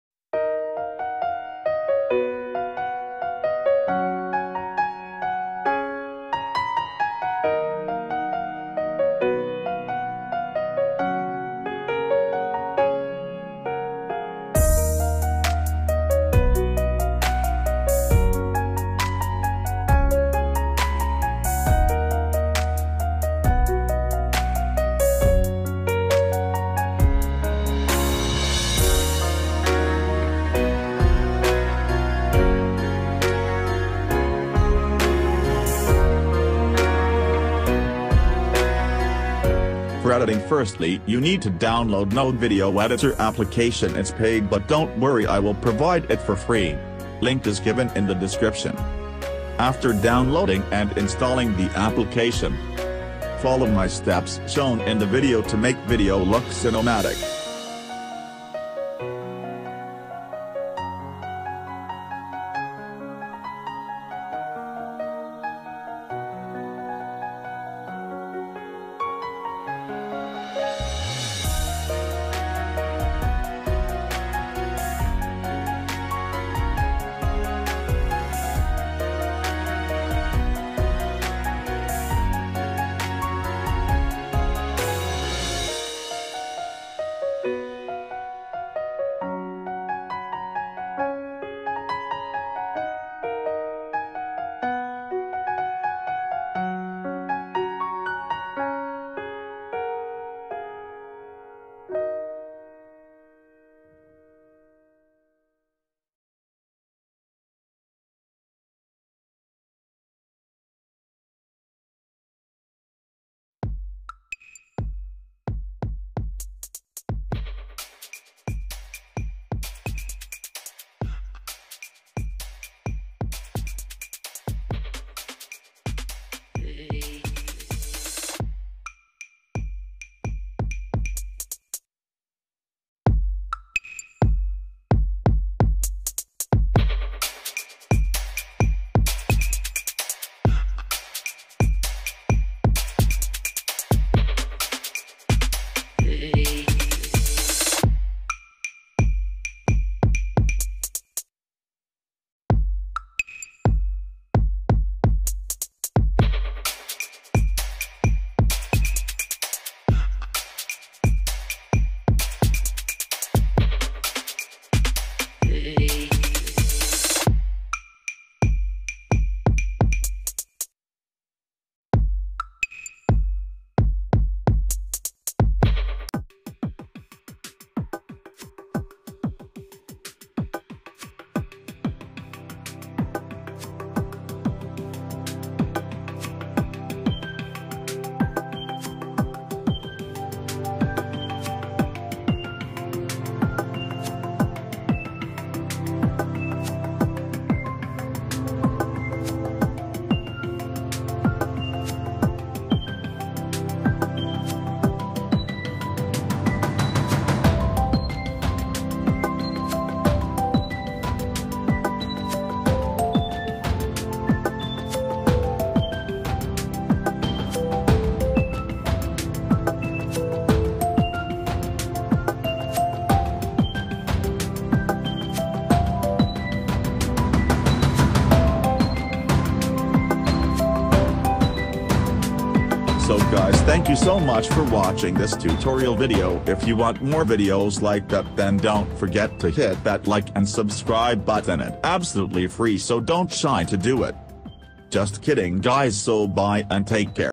Make normal video to cinematic looking video | Node video
In this tutorial video I gonna show how to edit your video to give it more cinematic look. I tried to show in easiest way I can. So hope uh learn something new from this video.